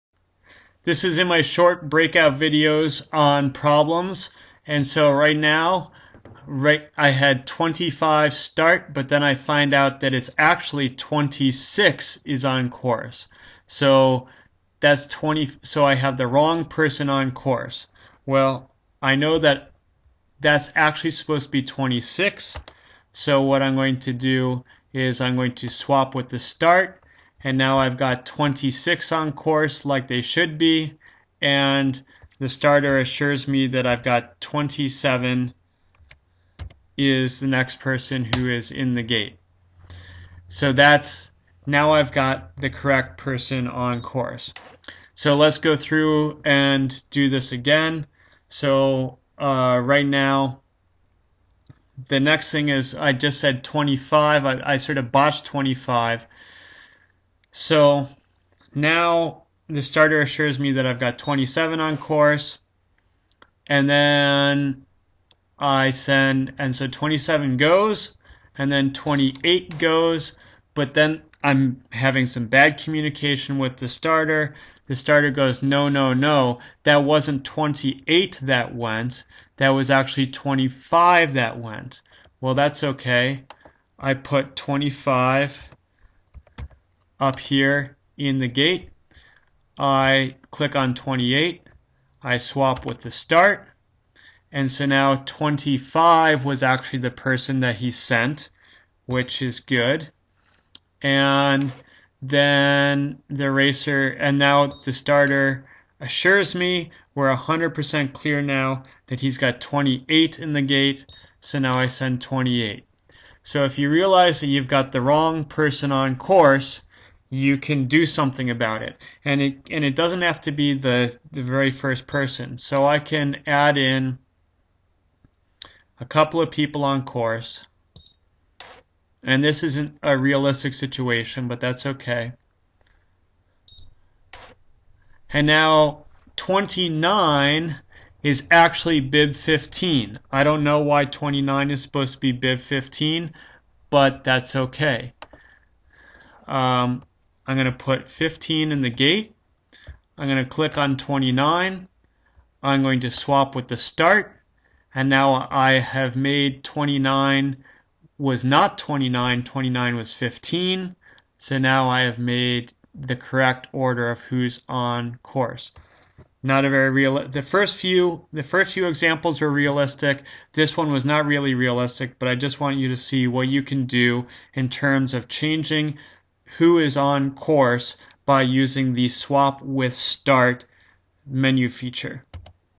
SwapWithStart
This video looks at how to use the Swap With Start feature in Split Second to fix some common problems that can arise while timing ski races.